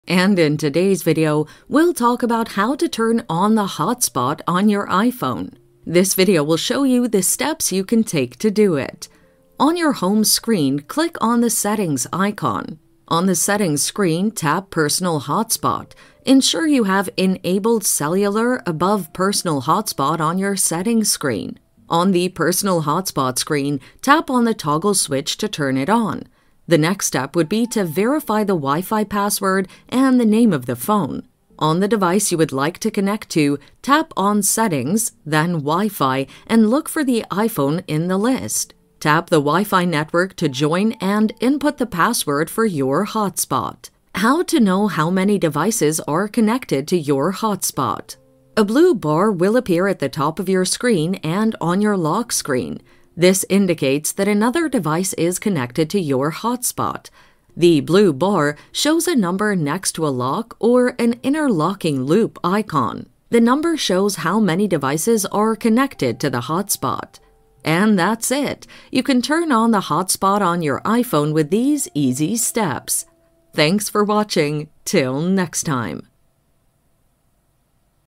How To Turn On The Hotspot On An iPhone (How To Set Up Hotspot on Your iPhone)
In this tutorial, we will show you how to turn on the hotspot on your iPhone.

On your home screen, click on the settings icon. On the settings screen, tap 'Personal Hotspot.’ Ensure you have enabled ‘Cellular’ above ‘Personal Hotspot' on your settings screen. On the 'Personal Hotspot' screen, tap on the toggle switch to turn it on. The next step would be to verify the Wi-Fi password and the name of the phone.

On the device you would like to connect to, tap on ‘Settings’ then ‘Wi-Fi’ and look for your iPhone in the list. Tap the Wi-Fi network to join and input the password for your hotspot.

How to know how many devices are connected to your hotspot
A blue bar will appear at the top of your screen and on your lock screen. This indicates that another device is connected to your hotspot. The blue bar shows a number next to a lock or interlocking loop icon. The number shows how many devices connected to the hotspot.